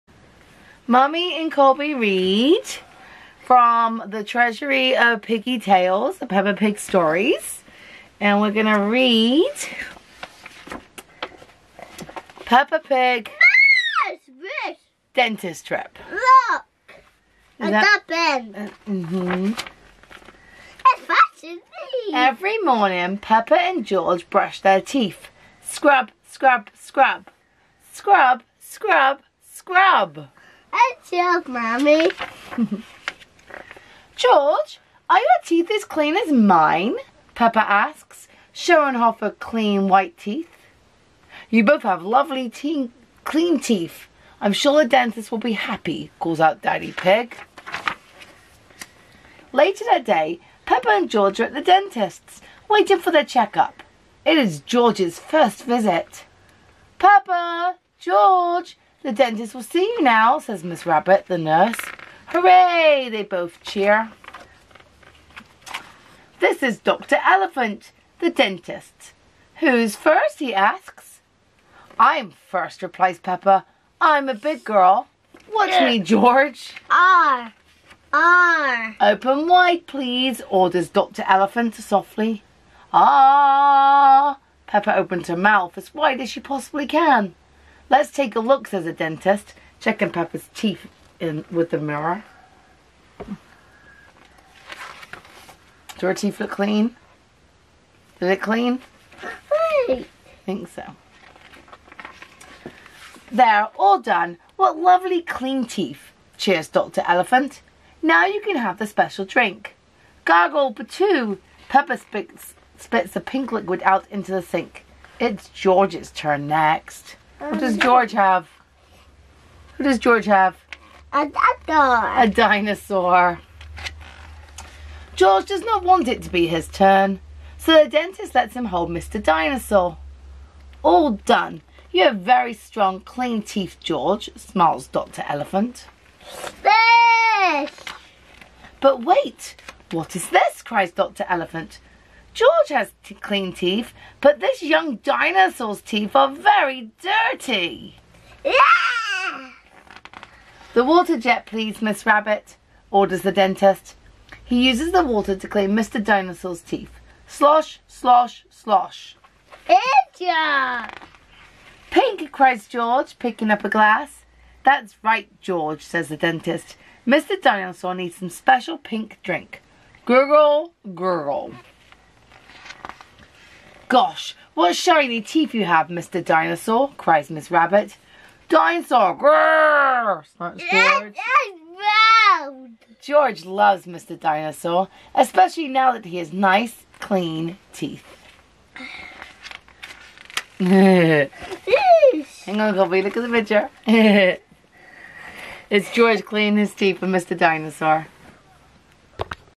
Mummy and Colby read from the Peppa Pig Treasury: Dentist Trip
Join us as we read one of Colby's most loved stories.